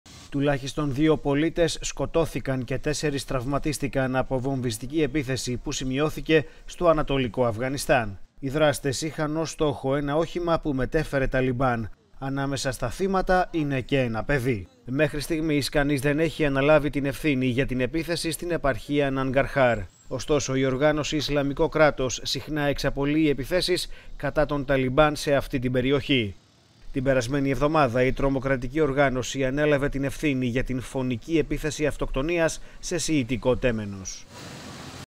Αφγανιστάν: Βομβιστική επίθεση με νεκρούς και τραυματίες
Μέχρι στιγμής καμία οργάνωση δεν έχει αναλάβει την ευθύνη